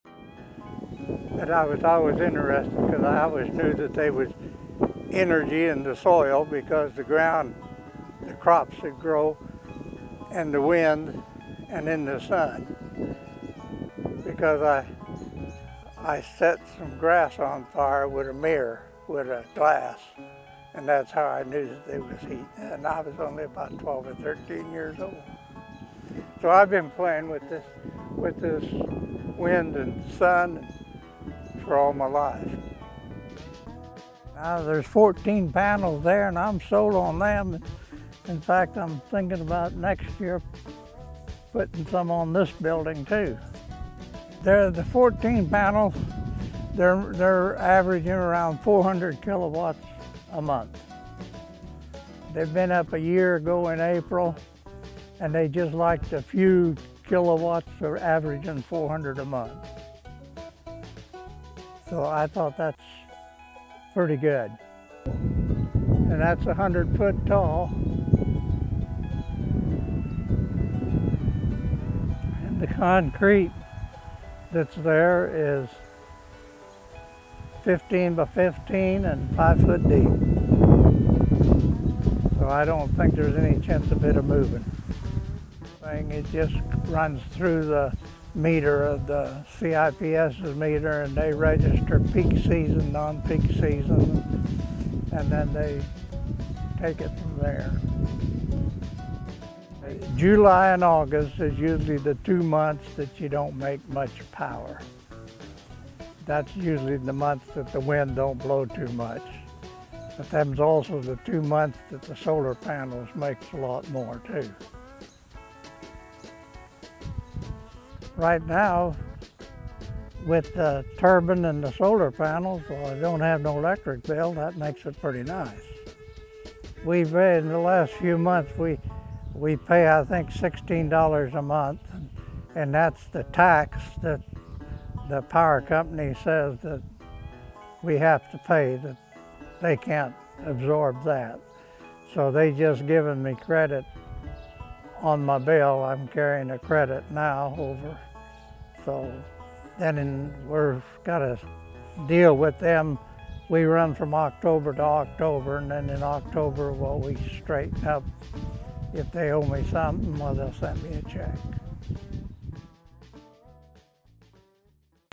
Bergey Wind Turbine and Solar PV System for Central Illinois Farm Home
Farmer Jim Macari of rural Shelbyville IL talks about his Bergey wind turbine and solar PV system and the installation completed by Tick Tock Energy, Inc.